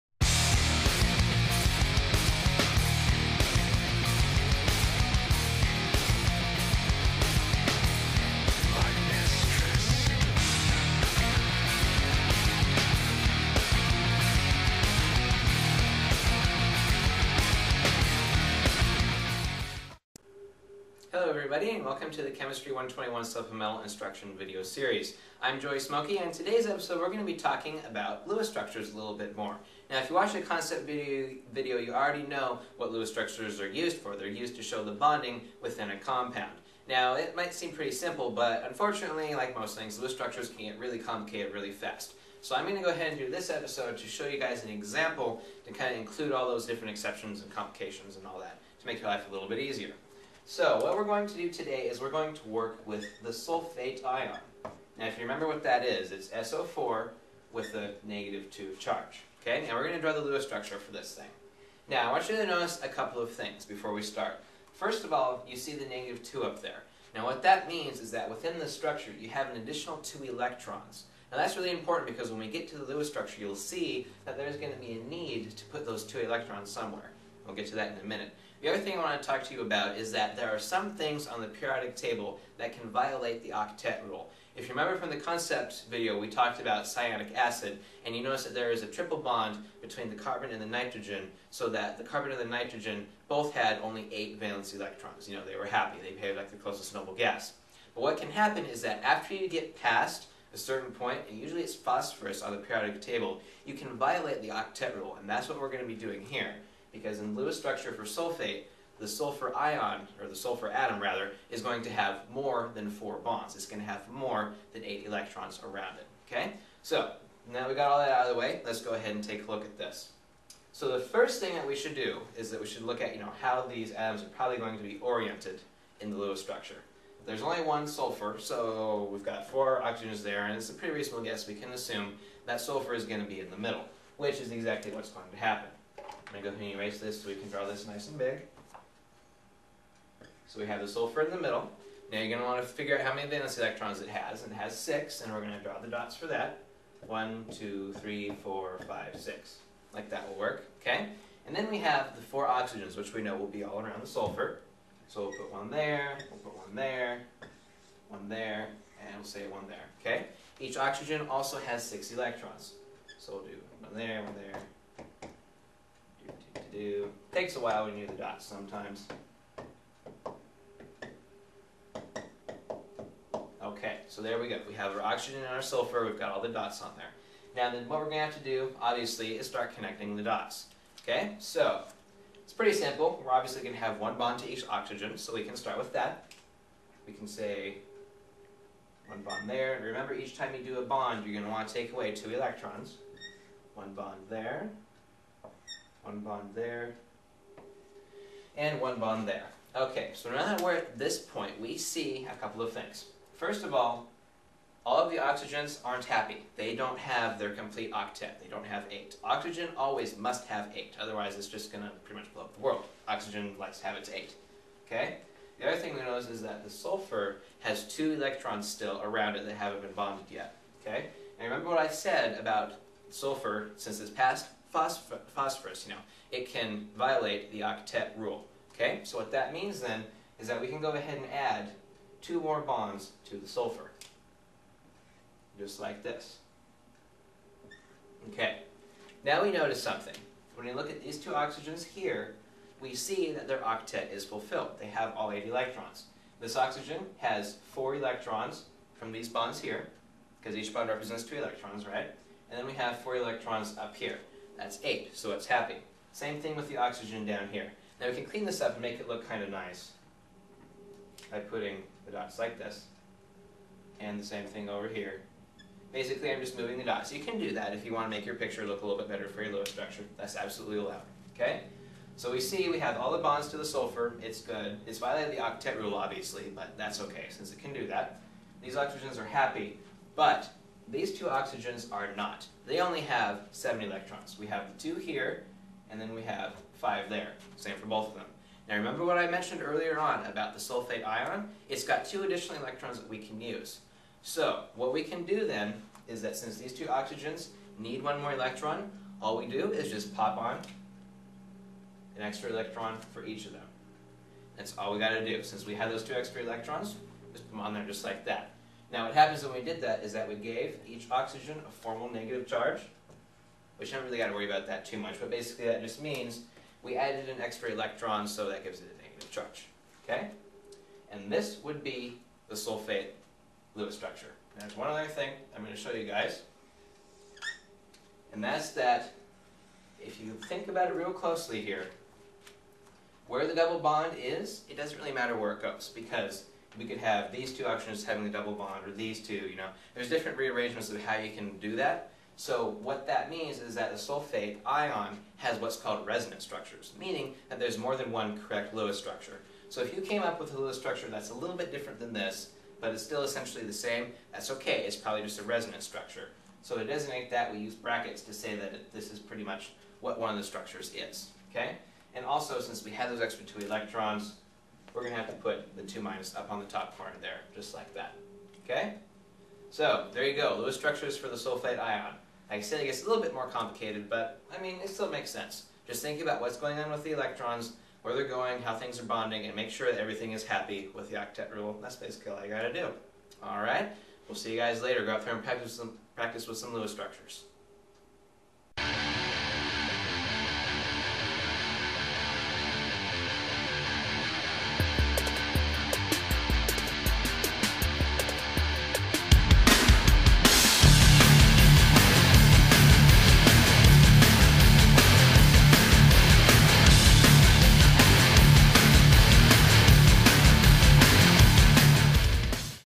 Lewis Structures Advanced Concept
Clark College Tutoring and Writing Center tutor Joey Smokey explains Lewis Structures in a more advanced fashion, introducing how certain elements break the octet rule, how to deal with ions in a structure, and what resonance structures are.